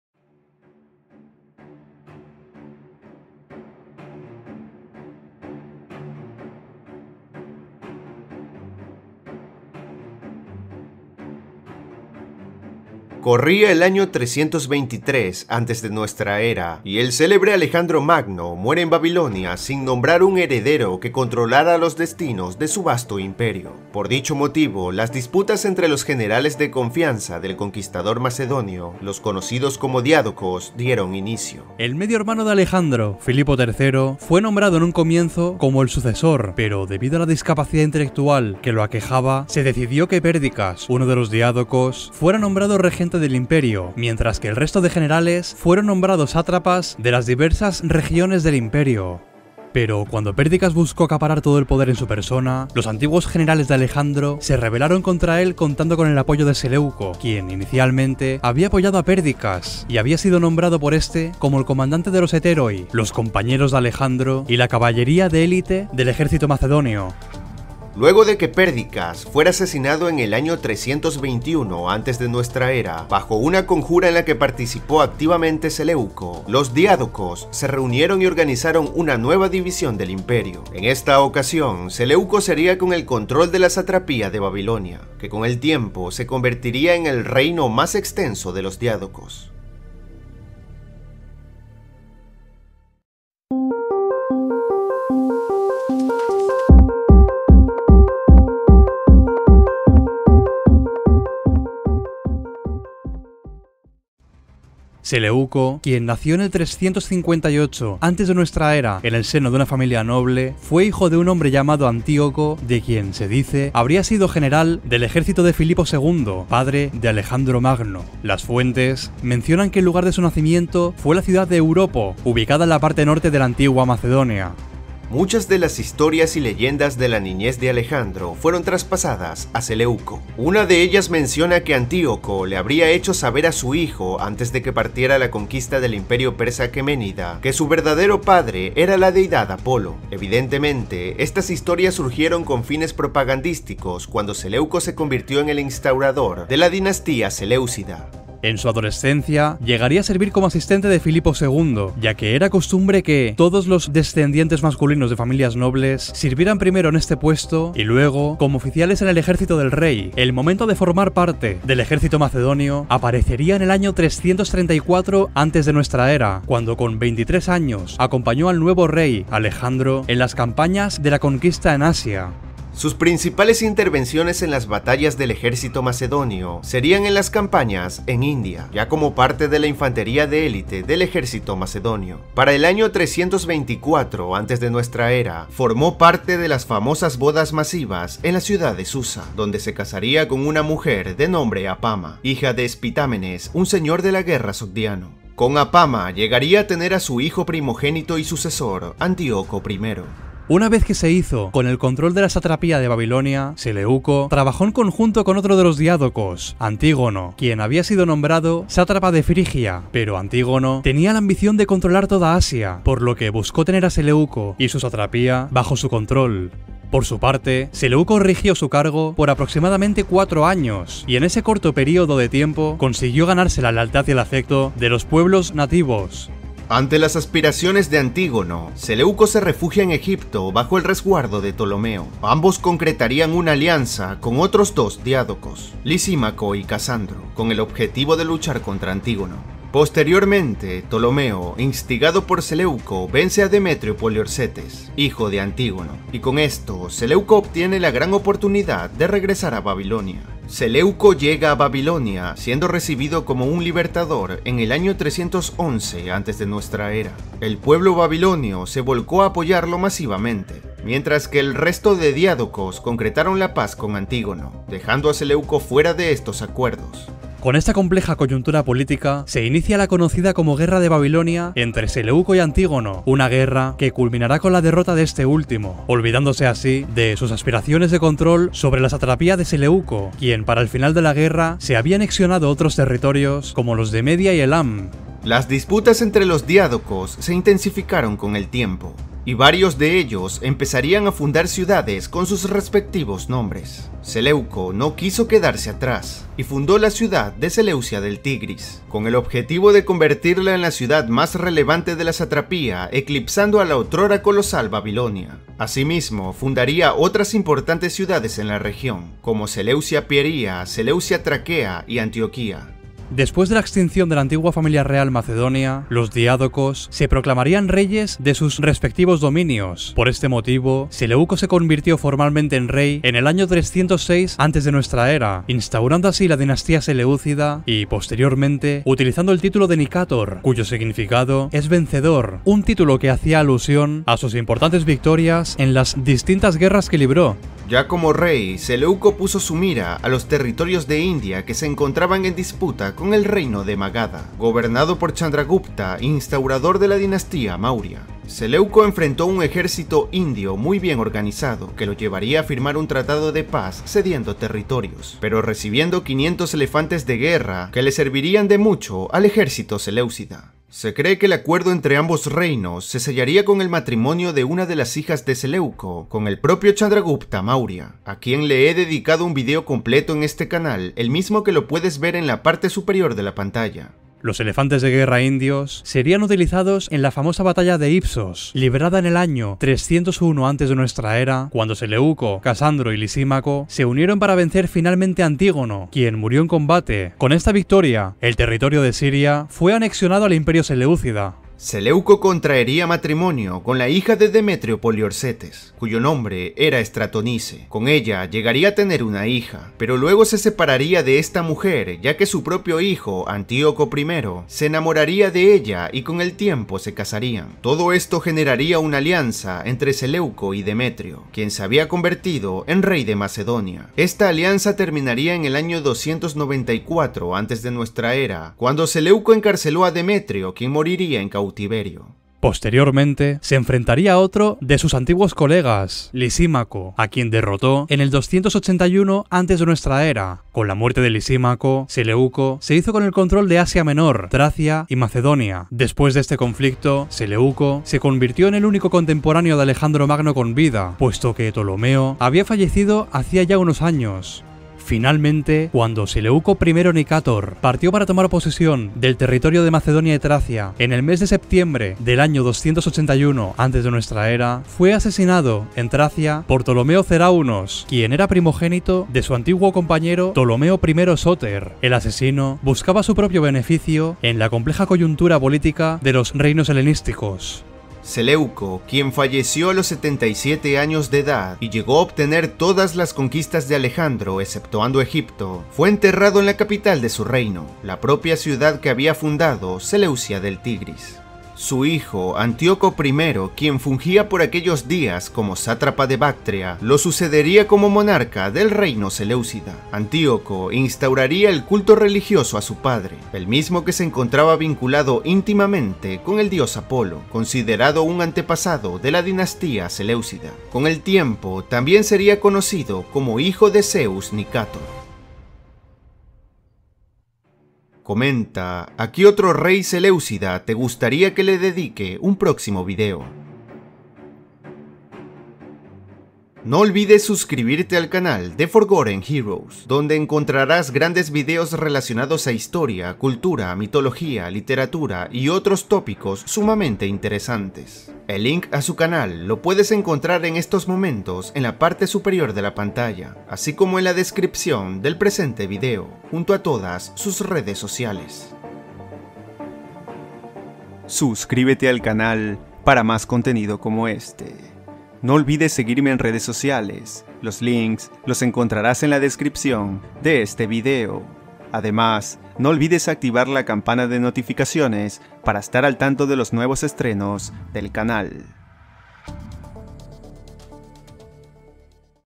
Seleuco I Nicátor - El Nacimiento del Imperio Seléucida 🏛️⚔️🔥 Colaboración con @ForgottenHeroes🎬
Corría el año 323 antes de nuestra era y el célebre Alejandro Magno muere en Babilonia sin nombrar un heredero que controlara los destinos de su vasto imperio. Por dicho motivo las disputas entre los generales de confianza del conquistador macedonio (los conocidos como diádocos) dieron inicio.
El medio hermano de Alejandro, Filipo III fue nombrado en un comienzo como el sucesor, pero debido a la discapacidad intelectual que lo aquejaba se decidió que Pérdicas (uno de los diádocos) fuera nombrado regente del imperio. Mientras que el resto de generales fueron nombrados sátrapas de las diversas regiones del Imperio.
Cuando Pérdicas buscó acaparar todo el poder en su persona, los antiguos generales de Alejandro se rebelaron contra él contando con el apoyo de Seleuco, quien inicialmente había apoyado a Pérdicas y había sino nombrado por este como comandante de los "hetairoi" (los compañeros de Alejandro y la caballería de élite del ejército macedonio).
Luego de que Pérdicas fuera asesinado en el año 321 antes de nuestra era bajo una conjura en la que participó activamente Seleuco, los diádocos se reunieron y organizaron una nueva división del imperio. En esta ocasión, Seleuco se haría con el control de la satrapía de Babilonia que con el tiempo se convertiría en el reino más extenso de los diádocos.

⚠️Nota Aclaratoria: Este vídeo tiene fines informativos, no pretende generar ningún tipo de ofensa o de polémica.

📚Bibliografía Consultada:
✔️ El reino seléucida: 312 - 63 a.C. - Héctor Manuel Vázquez Dovale.
✔️El mundo griego después de Alejandro, 323 - 30 a.C. - Graham Shipley.
✔️Alejandro Magno - National Geographic.
✔️El mundo helenístico - Pierre Léveque.